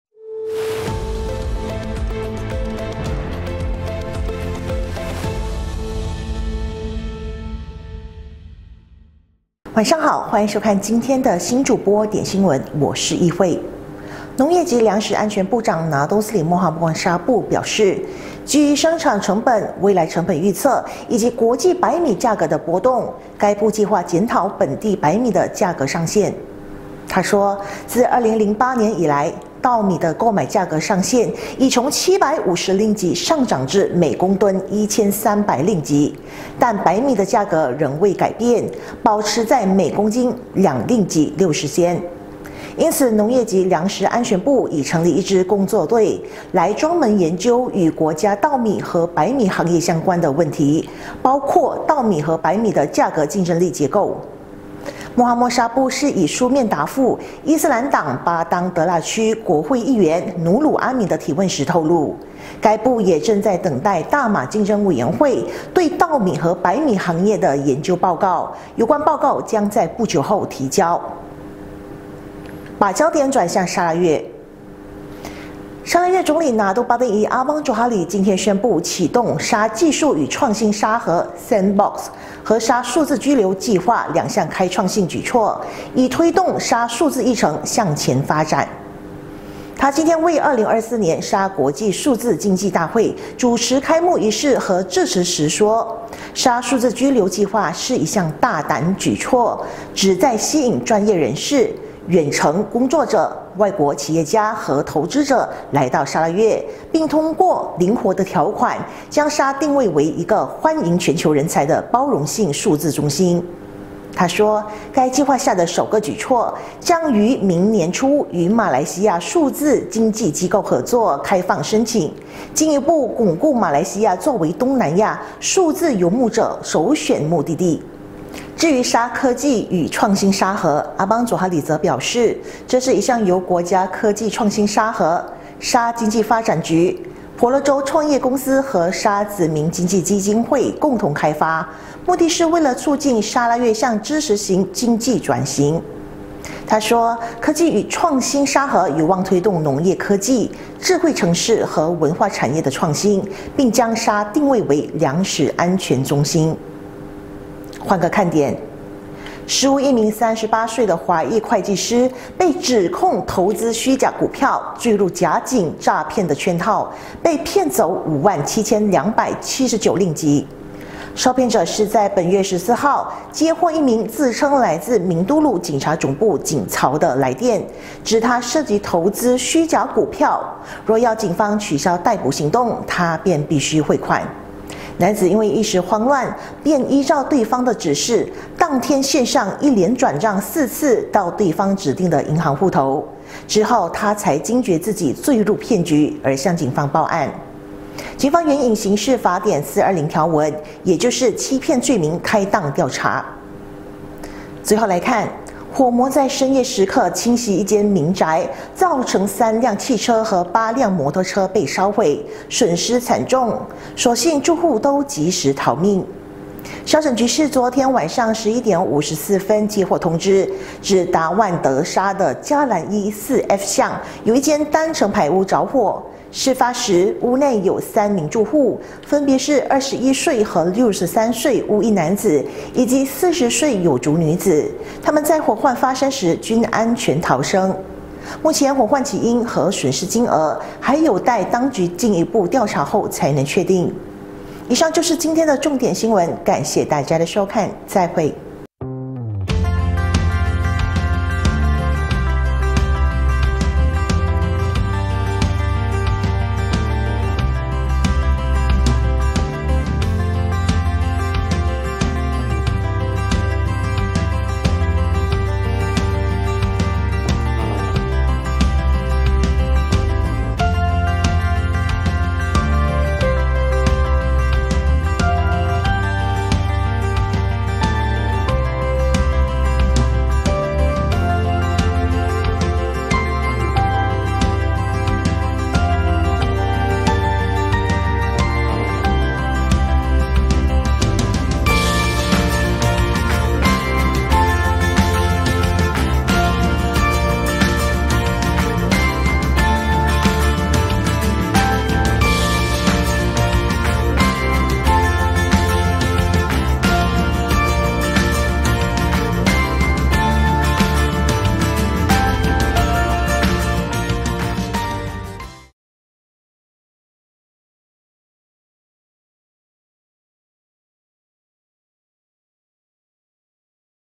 星主播点新闻 10月16日
★白米16年没涨价 农长：是时候检讨了
★宣布启动2开创性举措 总理推进砂数字议程发展
★坠假警电话骗局 会计师被骗5.7万
★火魔夜袭民宅 烧毁3汽车8摩托